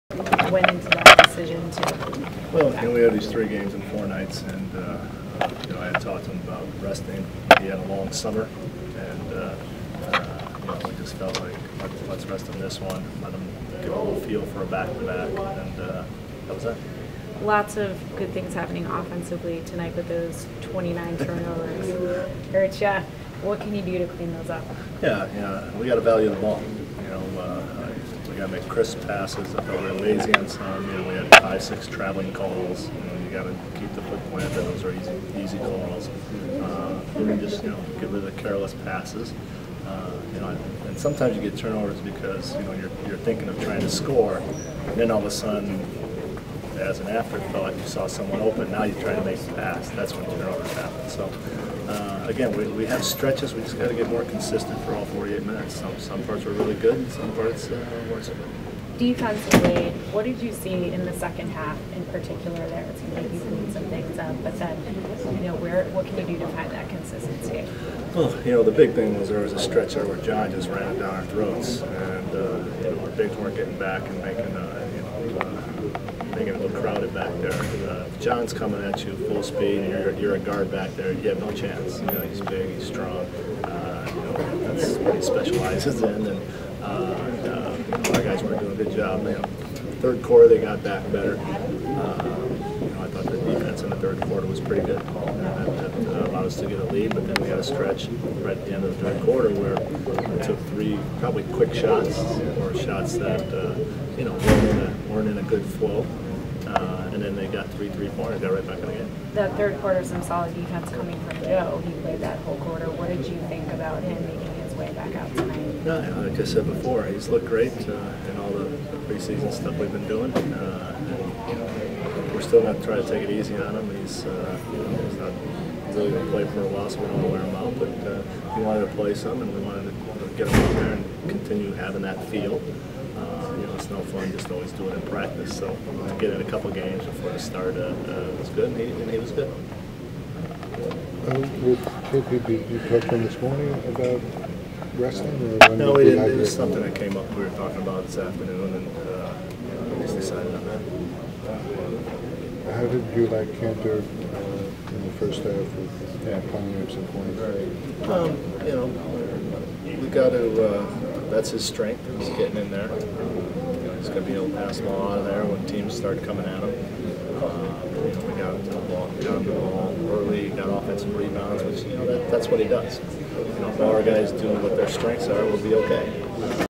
Washington Wizards vs New York Knicks - Knicks Coach Jeff Hornacek post game
Post game presser from Knicks head coach Jeff Hornacek  following Washington's preseason win over the Knicks.